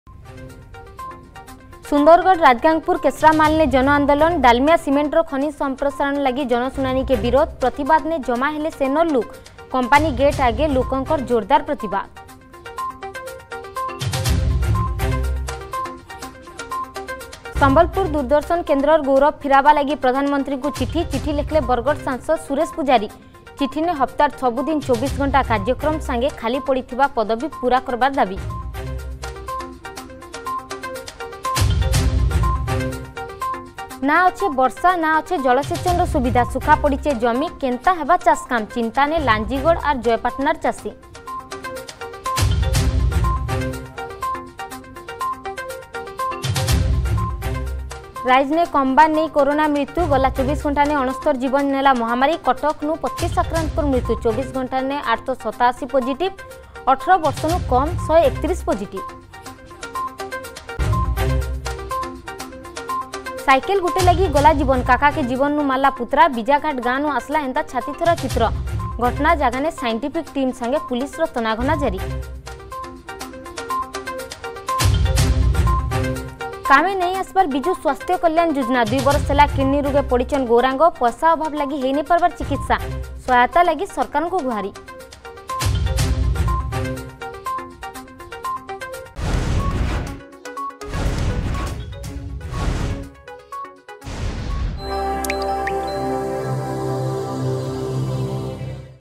2508 5 PM HL FB SRI
HEADLINE : 5PM
+ସୁନ୍ଦରଗଡ଼ ରାଜଗାଙ୍ଗପୁର୍ କେଶ୍ରାମାଲନେ ଜନ ଆନ୍ଦୋଳନ
+ସମ୍ବଲପୁର ଦୂରଦର୍ଶନ୍ କେନ୍ଦ୍ରର୍ ଗୌରବ ଫିରାବା ଲାଗି  ପ୍ରଧାନମନ୍ତ୍ରୀଙ୍କୁ ଚିଠି
+ନା  ଅଛେ ବର୍ଷା ନା ଅଛେ ଜଳସେଚନର ସୁବିଧା
+ରାଜ୍ୟରେ କମିବାର ନାଁ ନେଉନି କରୋନାମୃତ୍ୟୁ
+ସାଇକେଲ୍ ଗୁଟେ ଲାଗି ଗଲା ଜୀବନ୍
+କାମେ ନାଇ ଆସବାର୍ ବିଜୁ ସ୍ବାସ୍ଥ୍ୟ କଲ୍ୟାଣ ଯୋଜନା